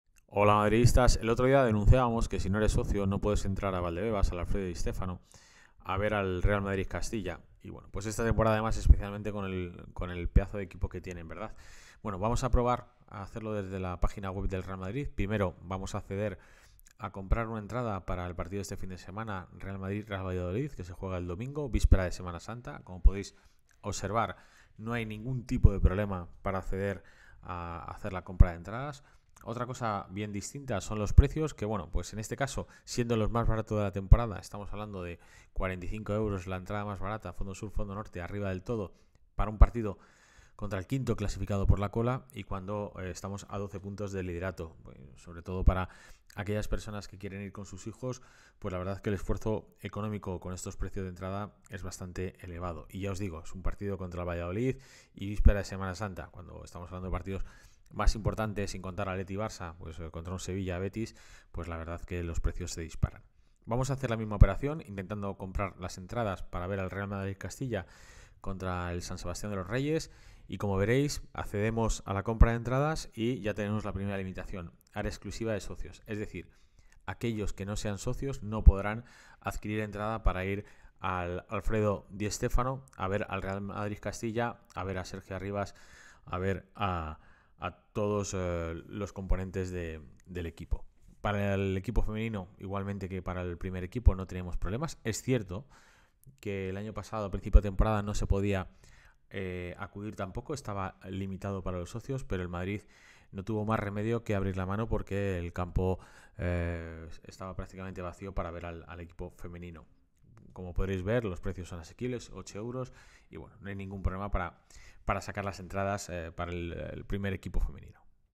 ¡¡¡IMPOSIBLE!!! VER AL REAL MADRID CASTILLA, DE SERGIO ARRIBAS EN EL ALFREDO DI STÉFANO
Lo primero, animar a todo el mundo a ir a ver al Real Madrid Femenino, las chicas lo necesitan y como habéis podido ver en el video las entradas son muy asequibles.

En nuestra carta abierta a Florentino Pérez, le pedimos que se pueda acceder al Alfredo Di Stefano a ver al Castilla, como decíamos es un ambiente muy familiar y es una forma agradable de acercar al madridismo más jóvenes a ver a las promesas.